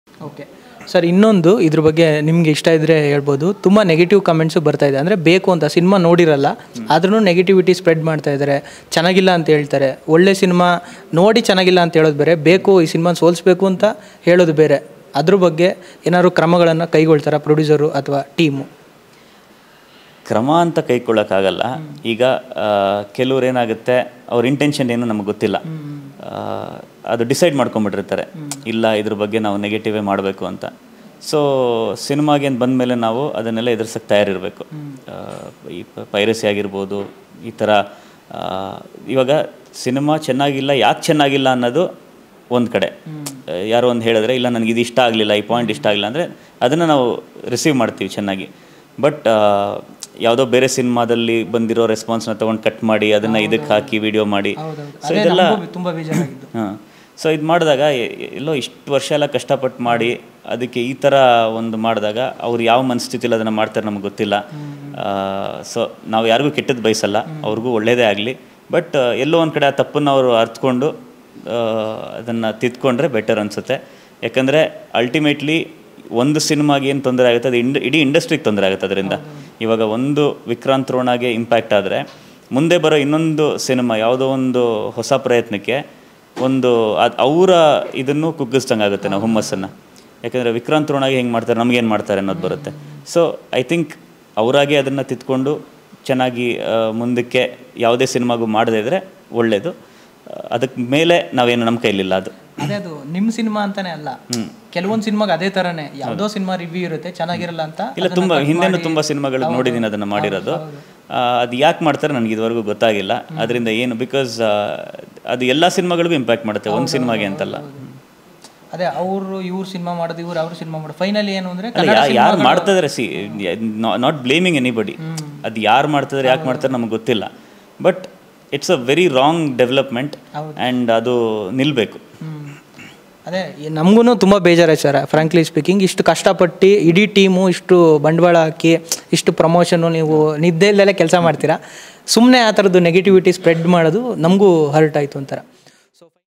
ಫೇಕ್ ರಿವ್ಯೂ ಮಾಡೋರಿಗೆ ಅನೂಪ್ ಖಡಕ್ ಉತ್ತರ | Anup Bhandari Interview | Vikrant Rona | Kichcha Sudeep
My Accounts